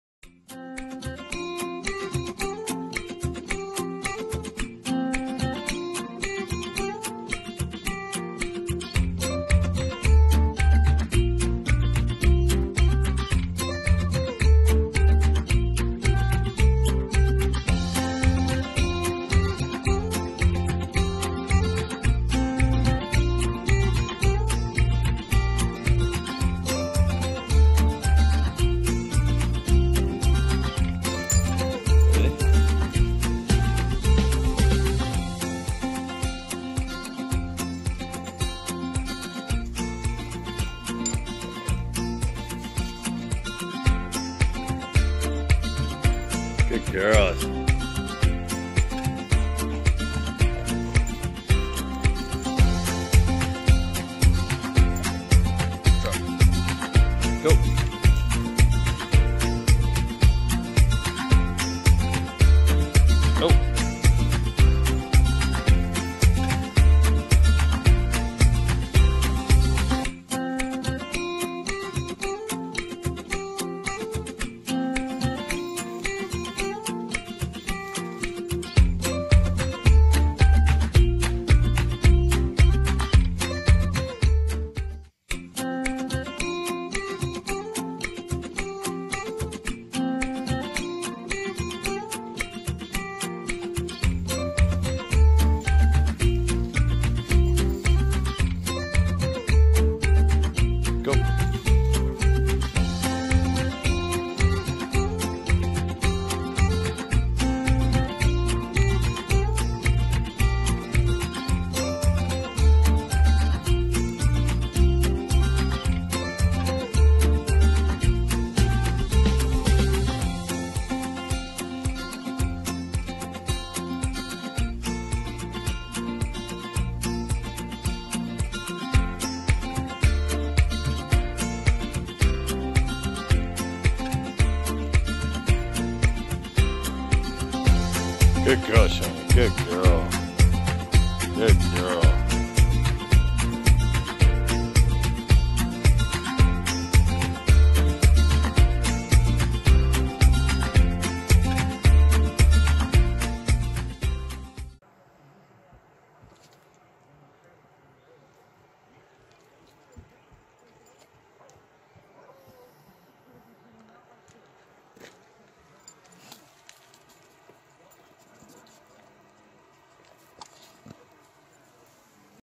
Shonie labrador retriever.. searching in the Woodlands..
Shonie and gunner travel around in there R.V,taking video and pictures of their travels, join in with likes and subscribes, and see where there adventures take you, enjoy,